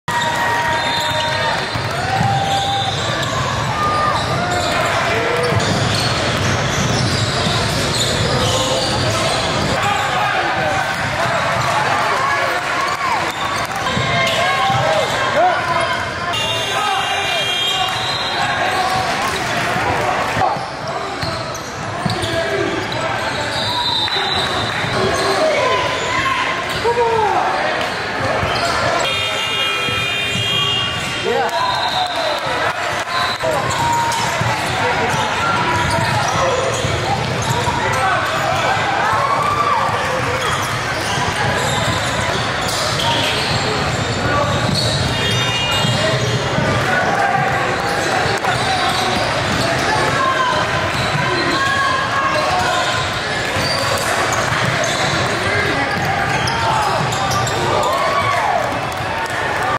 Gabriel Espinosa SHOOTING LIGHTS OUT for Shooters Elite at the 2021 Coca Cola Nationals
6'1" PG Gabriel Espinosa (2023) Varsity High School basketball player with Shooters Elite AAU club team at Coca Cola Nationals in So Cal averages 17 points per game and dishes out many assists. Gabe is a threat because he can catch and shoot or take defenders off the dribble, also does a great job moving without the ball...He's quick and hard to defend.